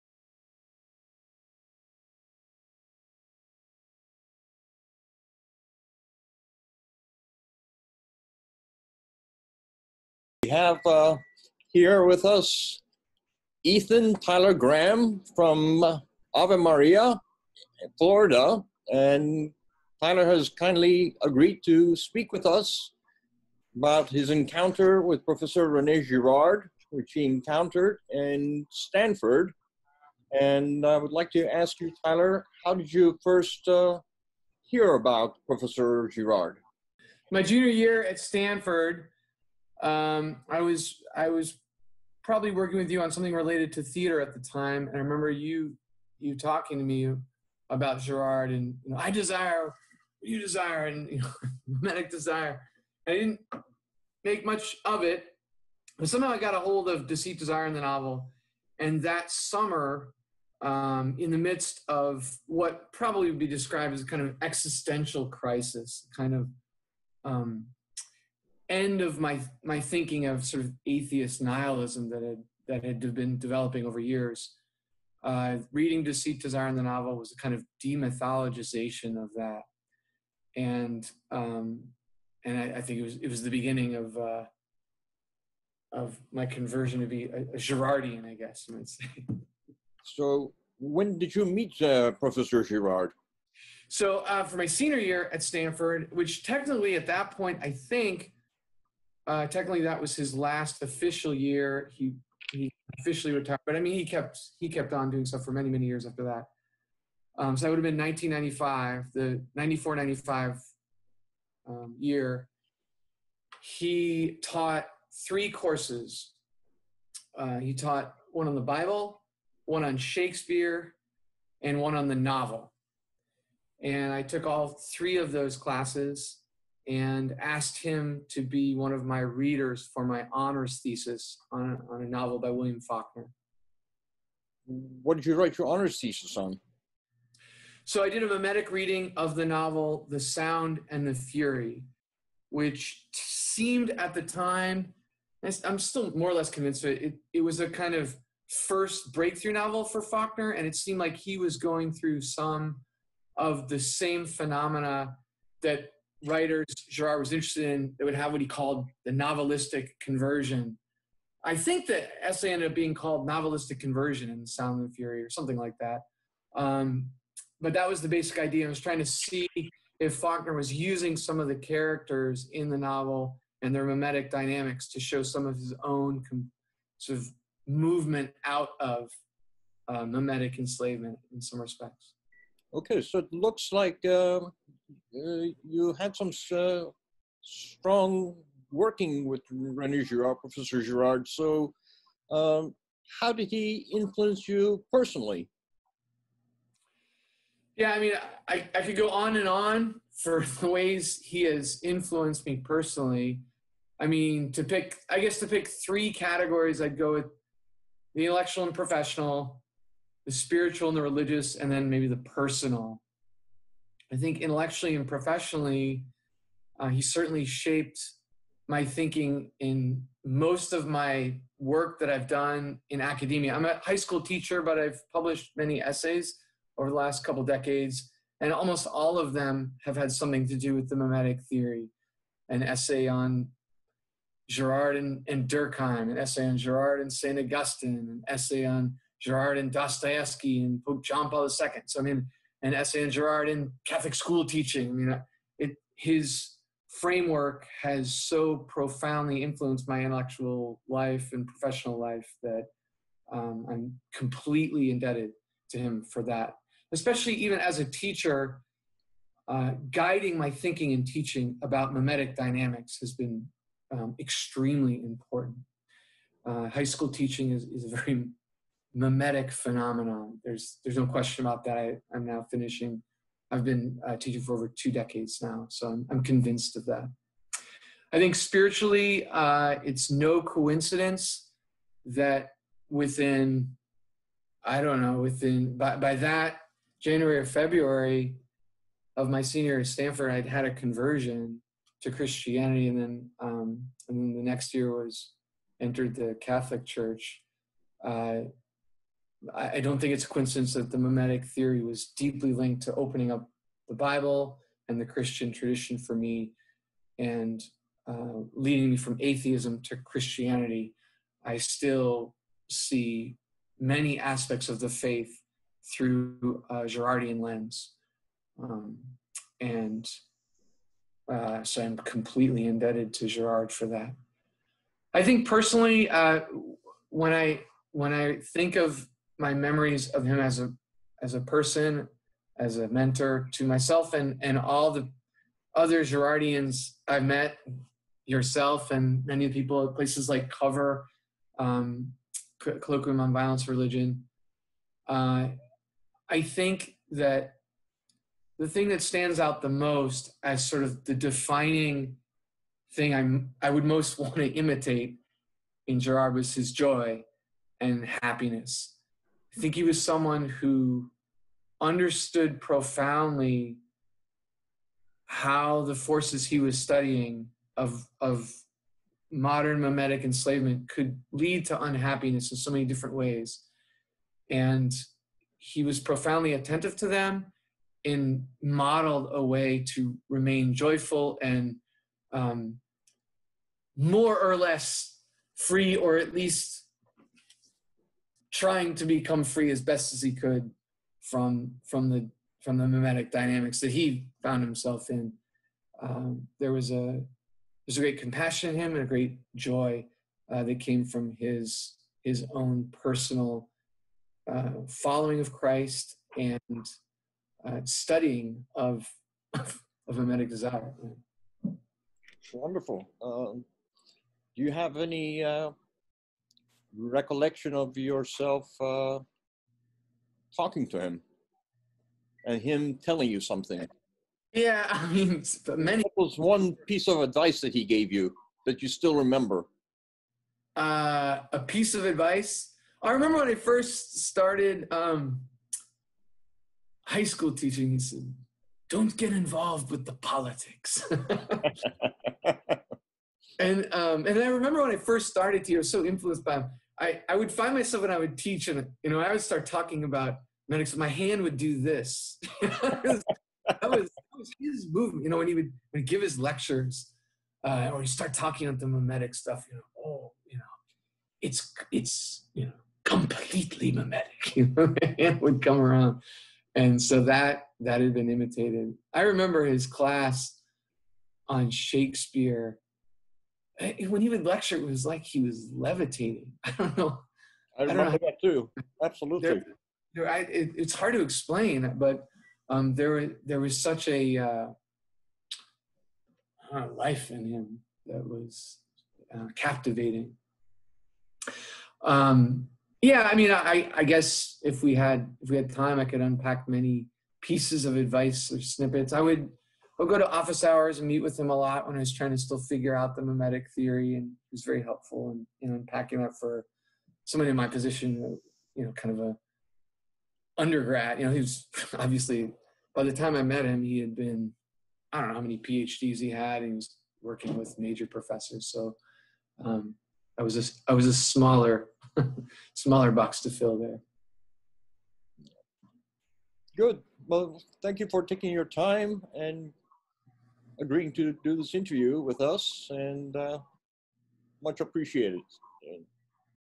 Tribute Film to René Girard by John Babak Ebrahimian with E. Tyler Graham
Part II. Stanford years

E. Tyler Graham has been teaching high school for over 20 years and has a humanities B.A. from Stanford University, an M.A. in religion from Syracuse, and a Master's in Theological Studies from Ave Maria University; he is currently completing a doctoral dissertation applying Girard’s ideas to a theology of the Catholic school teacher. He currently lives in Ave Maria, Florida, with his wife and 6 children. There, both spouses teach at — and all 6 children attend — Donahue Academy, a Catholic classical school.